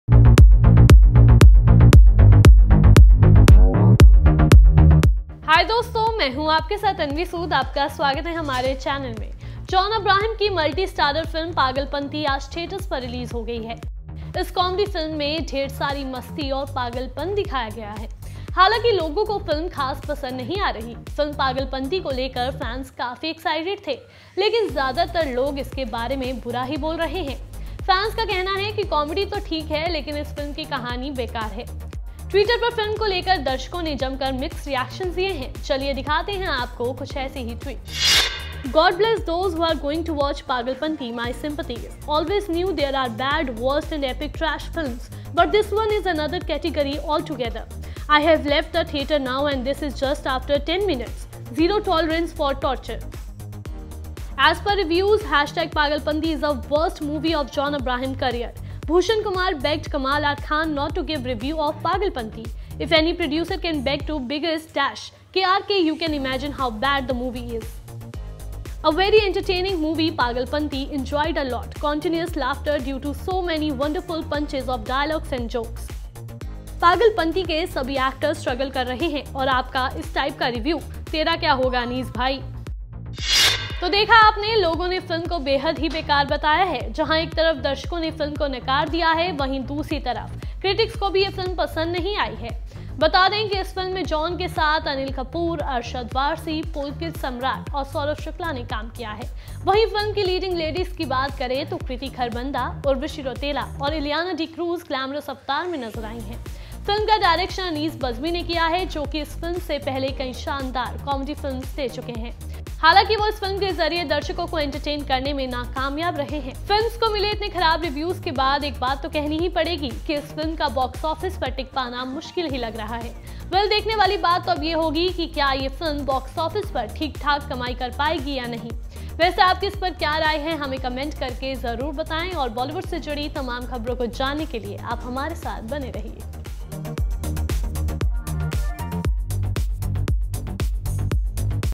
John की Pagalpanti को देखने के बाद Fans ने Twitter पर दिए ऐसे Reactions
Pagalpanti पर Twitter Reaction

जॉन अब्राहम की मल्टीस्टारर फिल्म पागलपंती आज थियेटर्स पर रिलीज हो गई है. इस कॉमेडी फिल्म में ढेर सारी मस्ती और पागलपन दिखाया गया है. हालांकि लोगों को फिल्म खास पसंद नहीं आ रही है. फिल्म पागलपंती को लेकर फैंस काफी exicted थे, लेकिन ज्यादातर लोग इसके बारे में बुरा ही बोल रहे हैं...फैंस का कहना है कि कॉमेडी तो ठीक है लेकिन इस फिल्म की कहानी बेकार है. Twitter पर फिल्म को लेकर दर्शकों ने जमकर mixed reactions दिए हैं. चलिए दिखाते हैं आपको ऐसे ही कुछ Tweets...